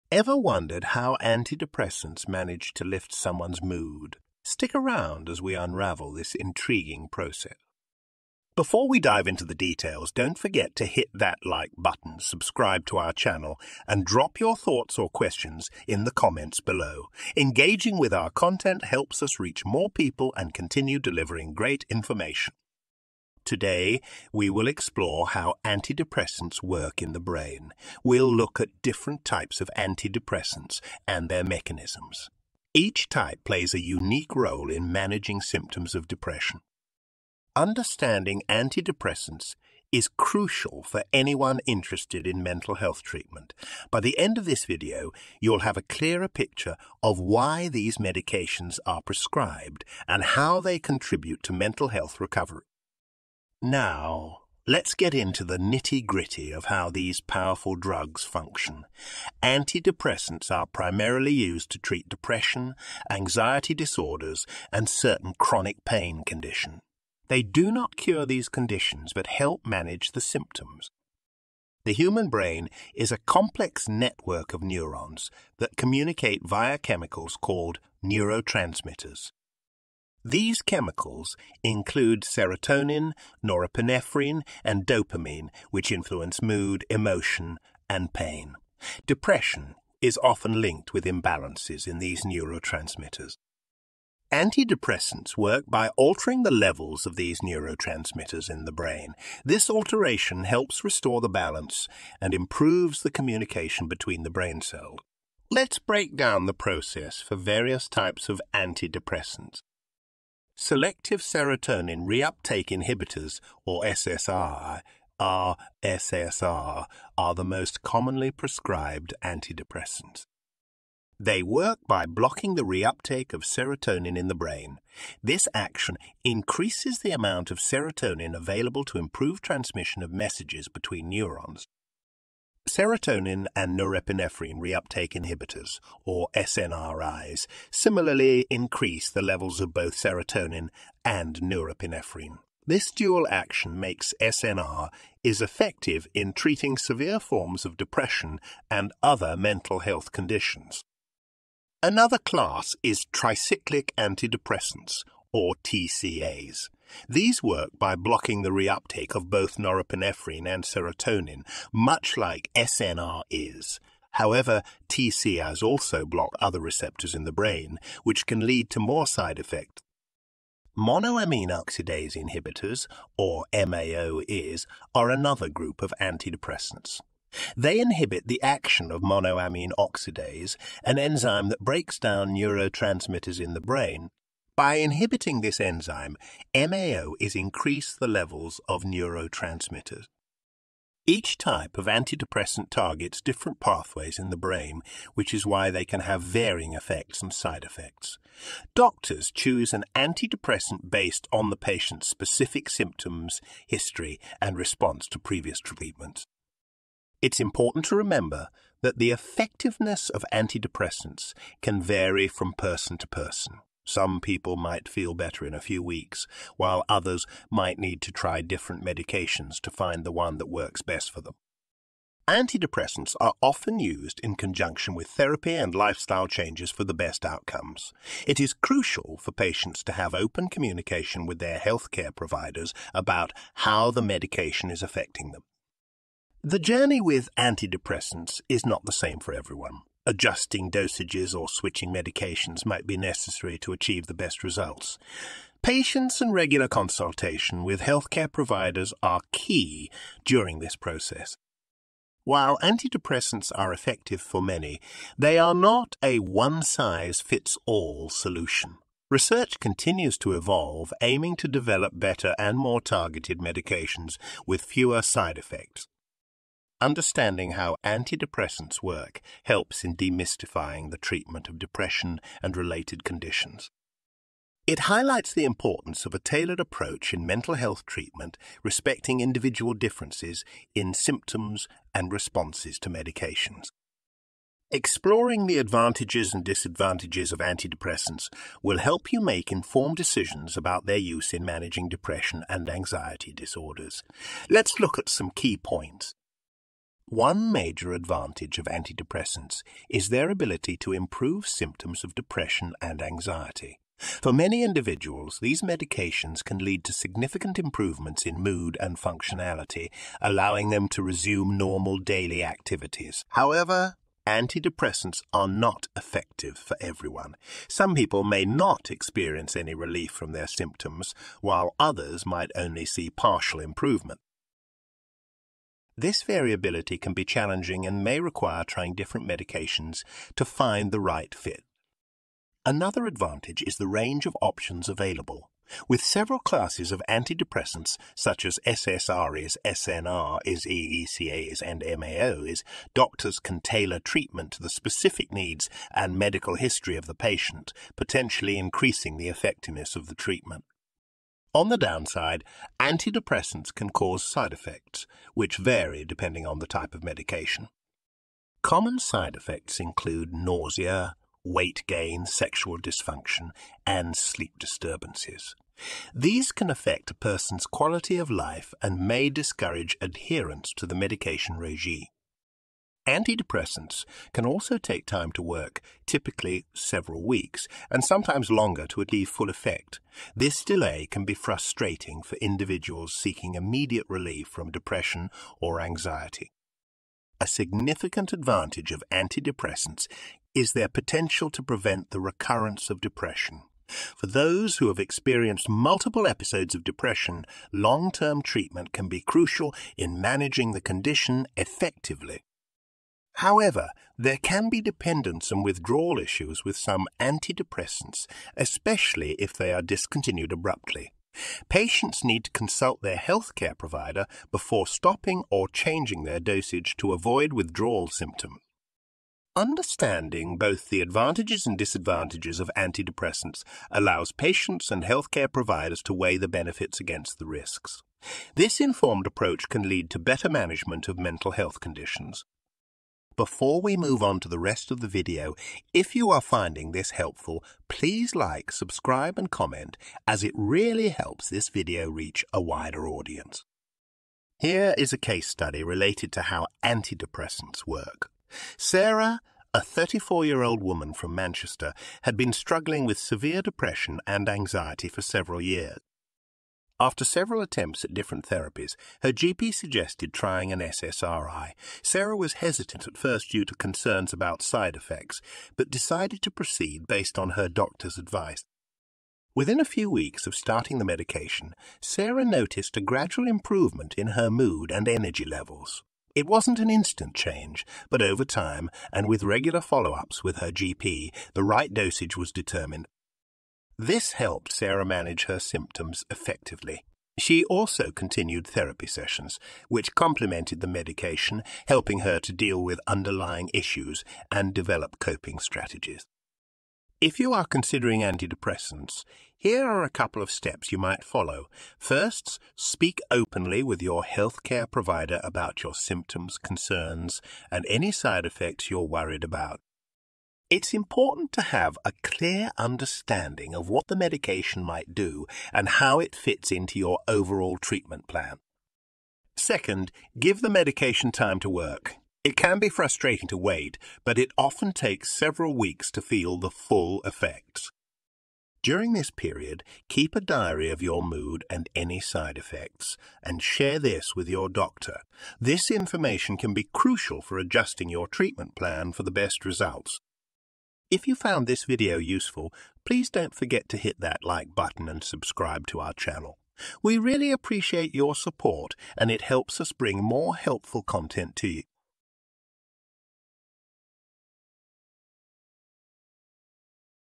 How Do Anti Depressants Work?
Ever wondered how a simple pill can change your mood and help you feel better? Discover the science behind anti-depressants in our latest video, "How do anti-depressants work?" This video offers a detailed explanation of the mechanisms through which medications like S S R I, S N R I, T C A, and M A O I help alleviate symptoms of depression and anxiety.

Anti-depressants are a cornerstone of modern mental health treatment, providing relief for millions worldwide. Understanding their function can empower those suffering from depression and anxiety by providing them with options for recovery. With insights into serotonin boosters and the effects of altering brain chemistry, viewers will gain a comprehensive overview of how these drugs can impact mental wellness.

The video also addresses common concerns about anti-depressants, such as potential side effects and the importance of proper management under healthcare supervision. It is crucial for anyone considering these medications to be informed about both the benefits and the risks involved.

Additionally, the video highlights the importance of a tailored approach in treating mental health issues, recognising that what works for one individual may not work for another. This personalised perspective on depression treatment and mental health support underscores the need for close cooperation between patients and healthcare providers.

By watching this video, viewers will not only learn about how anti-depressants work but also about the broader context of mental health therapy, including other supportive measures that can enhance recovery. This holistic view helps demystify the process and encourages a proactive approach to mental health care.

Actionable Insights on Managing Mental Health
For those exploring the option of anti-depressants or seeking to manage their mental health better, this video provides actionable advice and in-depth knowledge. It encourages viewers to engage actively with healthcare providers, seek comprehensive information, and consider a combined approach involving medication, therapy, and lifestyle adjustments for optimal health.